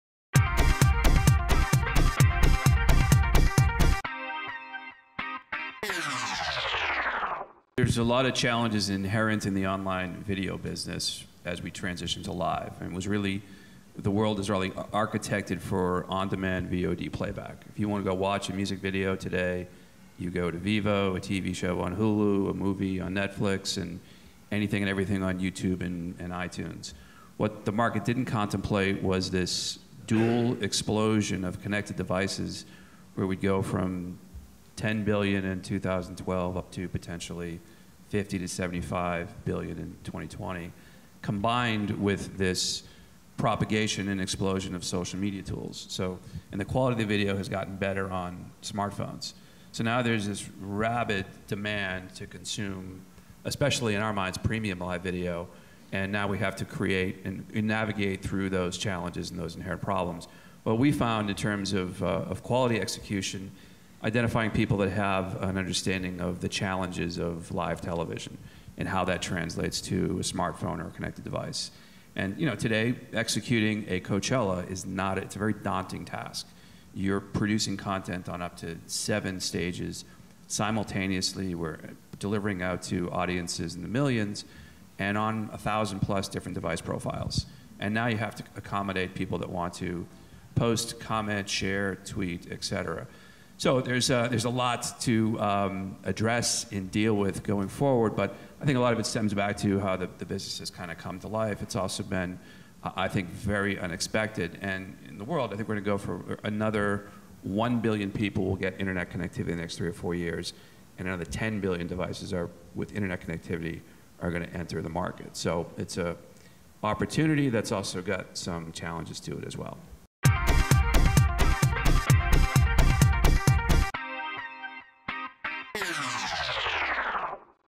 How Do You Translate the Lessons of Live TV to Smartphones and Connected Devices?
John Petrocelli explores the challenges of delivering a large-scale live event like Coachella to an audience looking for live interactivity and active engagement using an infrastructure built for on-demand viewing.